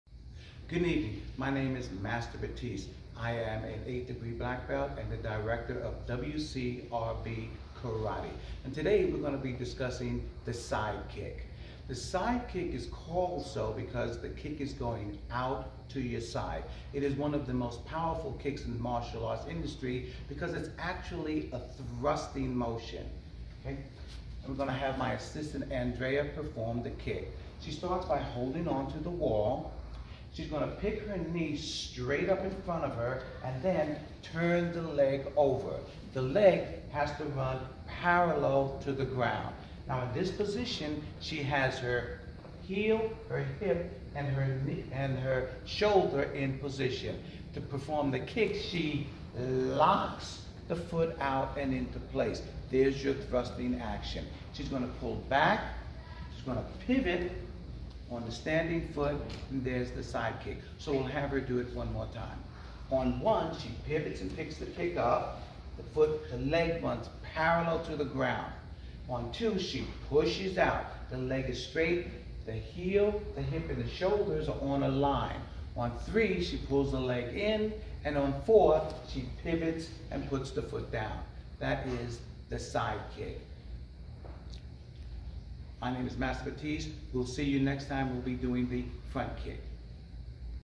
WCRB Karate - The Side Kick
WCRB Karate presents - The proper way of performing the Side Kick demonstrated by Master Rodney Batiste and Andrea